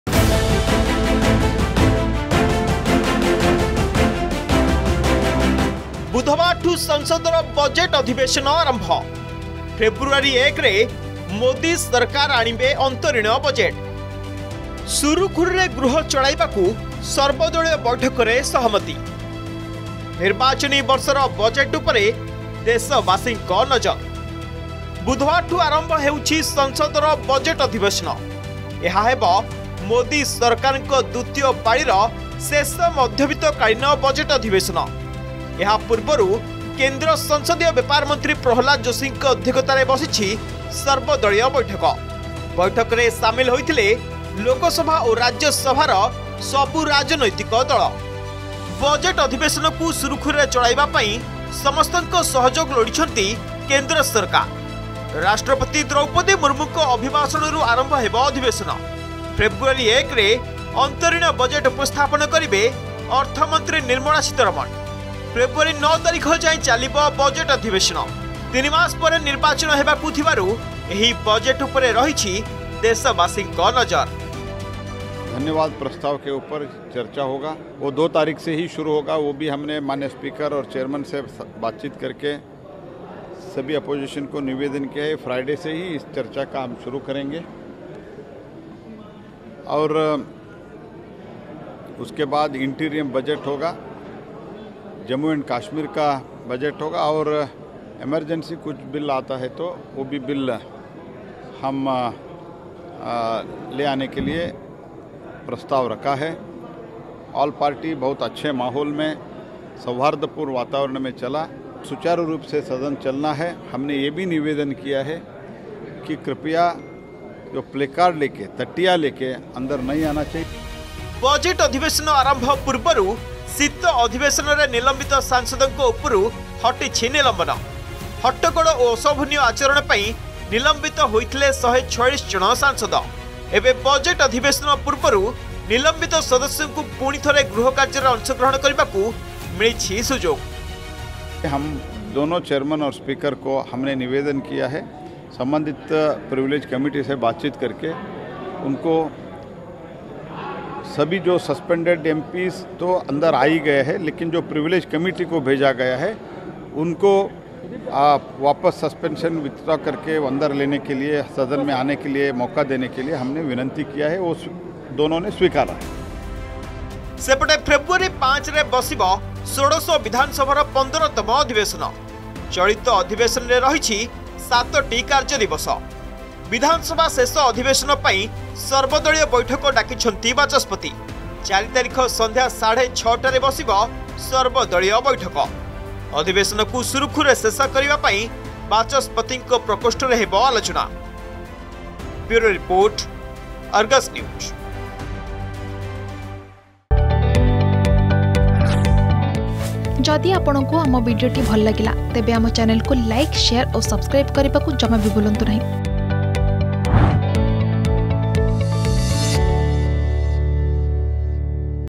ଫେବ୍ରୁଆରୀ ୧ରେ ମୋଦି ସରକାର ଆଣିବେ ଅନ୍ତରୀଣ ବଜେଟ୍ || Budget || Modi Govt
କାଲିଠାରୁ ସଂସଦର ବଜେଟ ଅଧିବେଶନ । ଫେବ୍ରୁଆରୀ ୧ରେ ଆସିବ ଅନ୍ତରୀଣ ବଜେଟ୍ । ମୋଦି ସରକାରଙ୍କ ଦ୍ବିତୀୟ ପାଳିର ଶେଷ ବଜେଟ ଉପରେ ଦେଶବାସୀଙ୍କ ନଜର ।